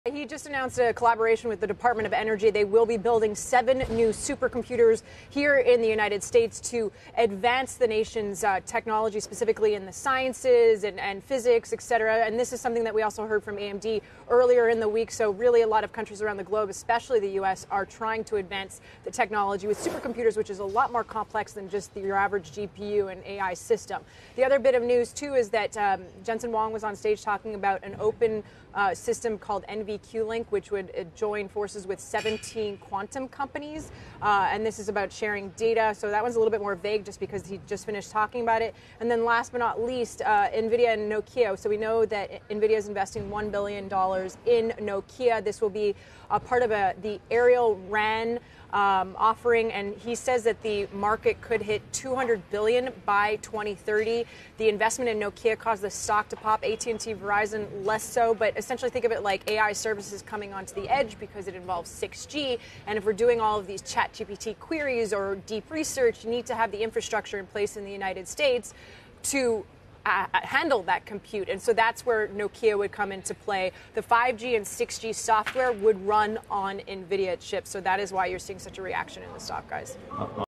Nvidia to partner with U.S. Dept. of Energy to build 7 new AI supercomputers
Nvidia CEO Jensen Huang announces during the company's GTC conference on Tuesday a partnership with the U.S. Department of Energy to build seven supercomputers.